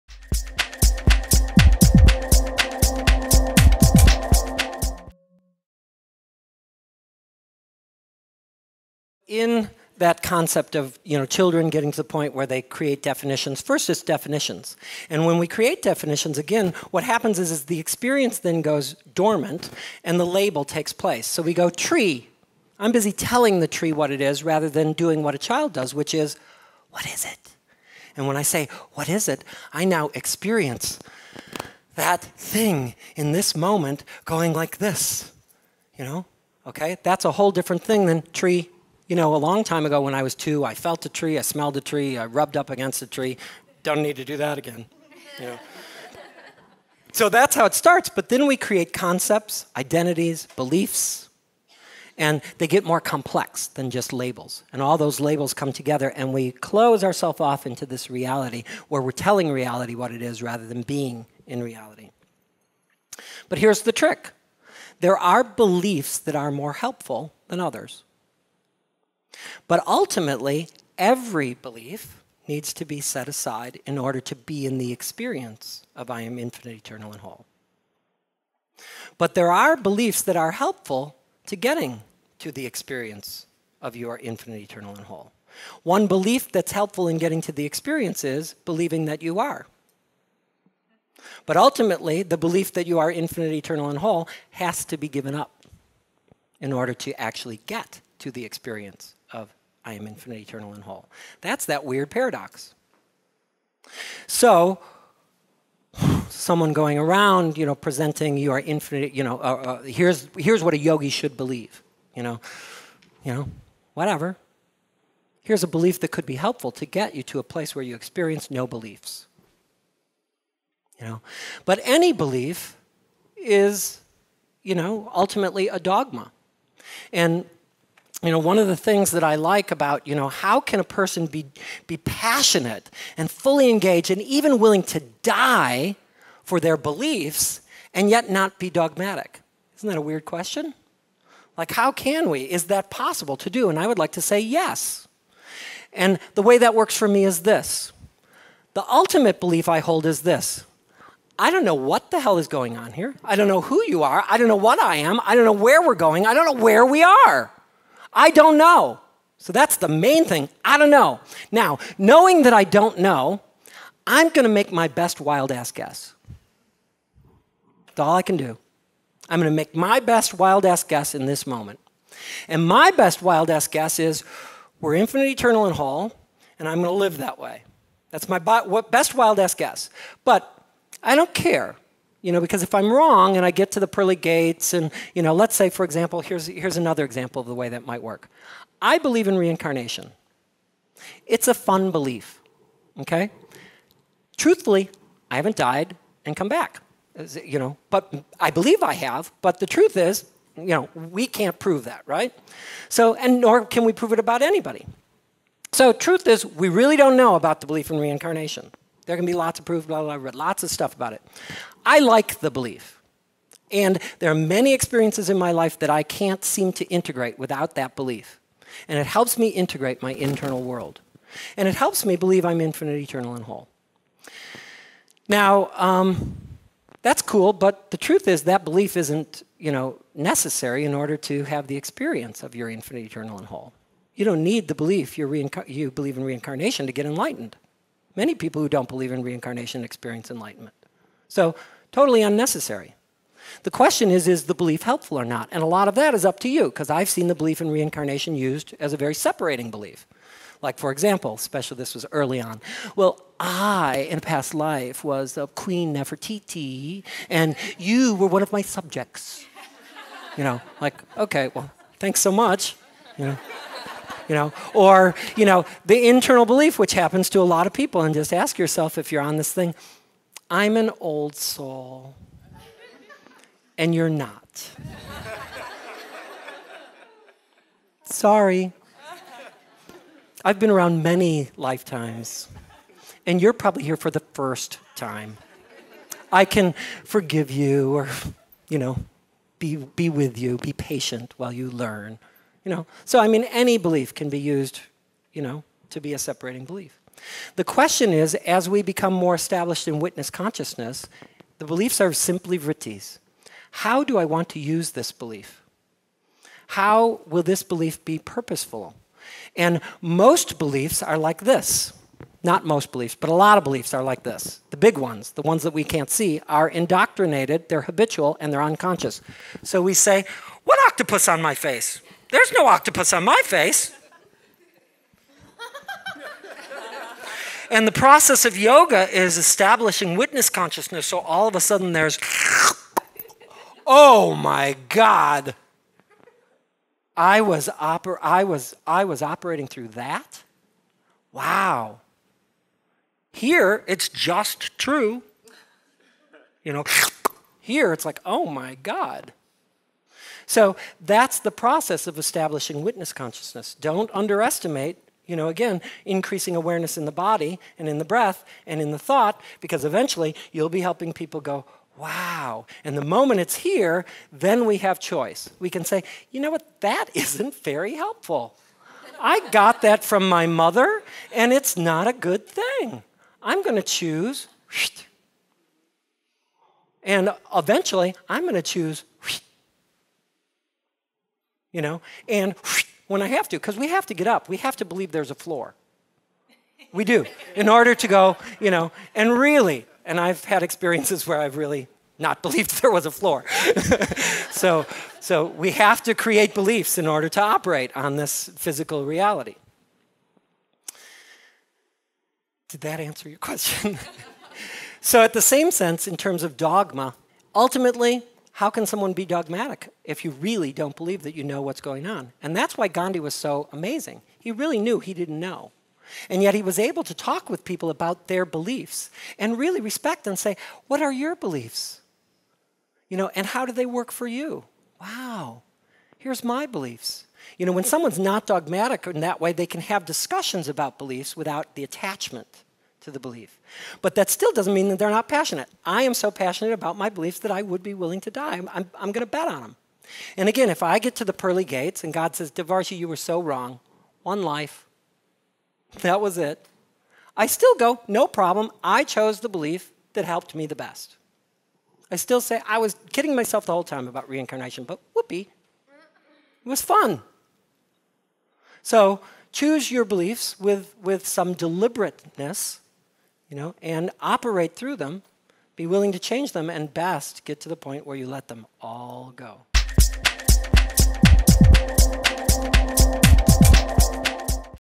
10. Gita Lecture - Beliefs
The tenth part of a two-hour discussion on the Bhagavad Gita with Devarshi Steven Hartman.  Founder of the Pranotthan Yoga School.  Former Kripalu Ashram Resident and former dean of the Kripalu Yoga School 2004-14.